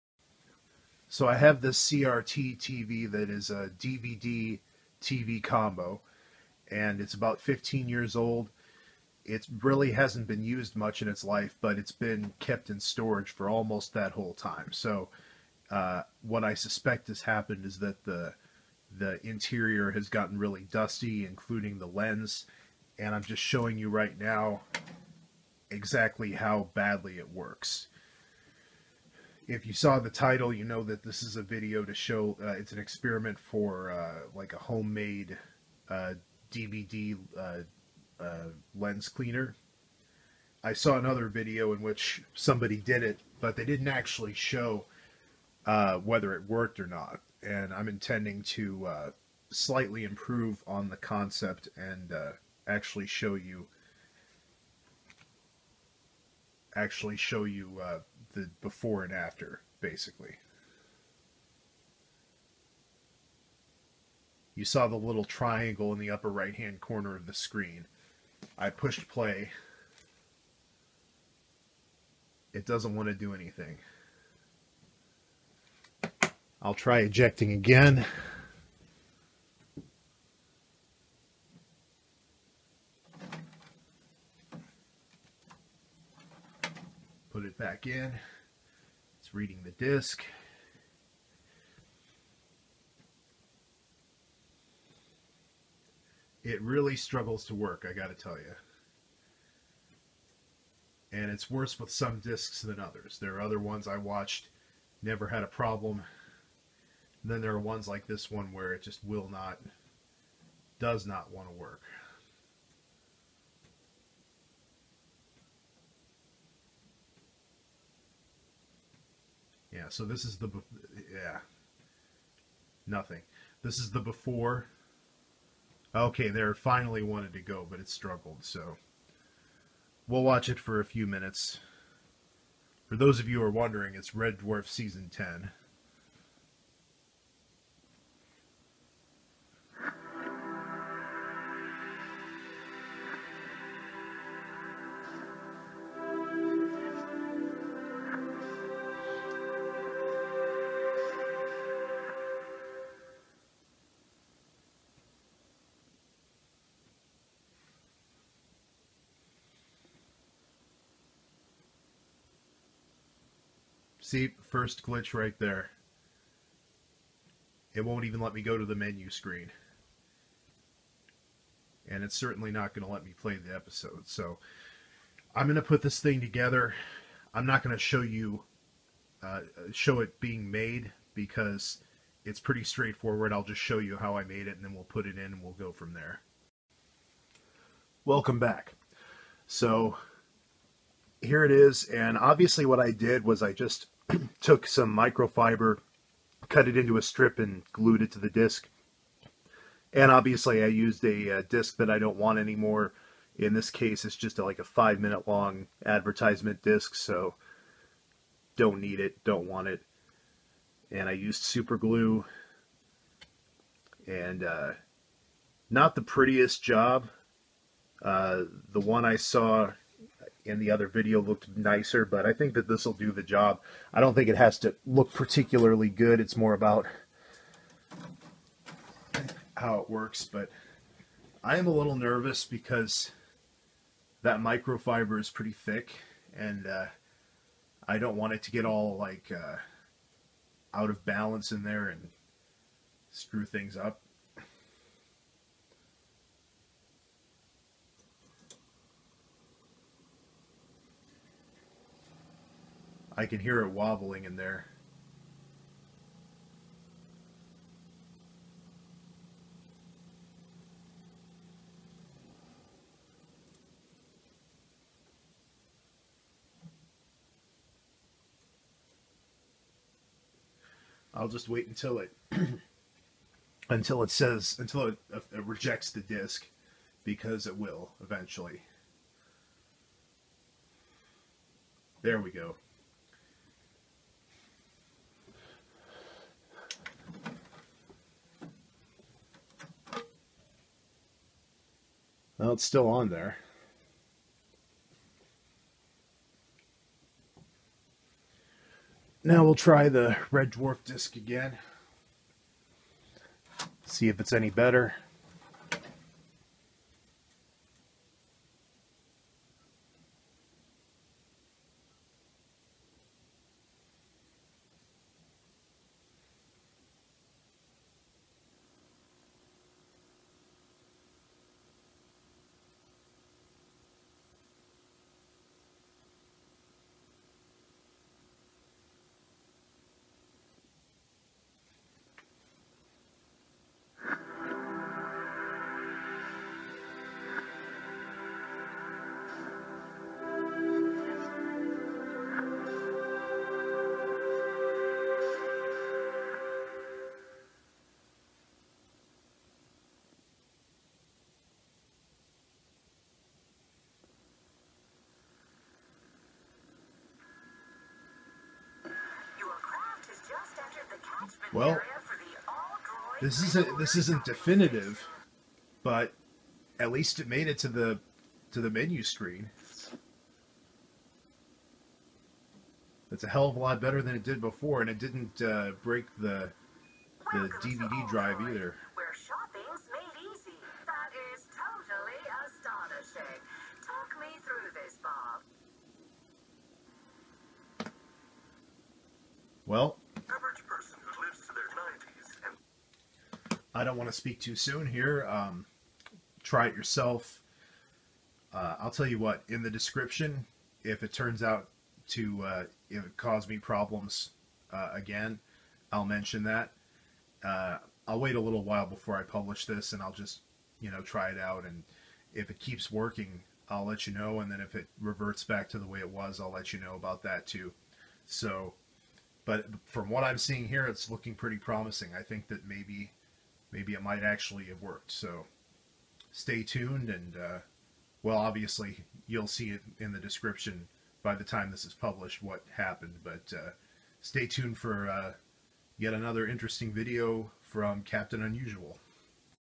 Homemade Lens-Cleaning DVD (Experiment)
Although this experiment seemed to work for me, I would advise extreme caution if you decide to attempt this yourself.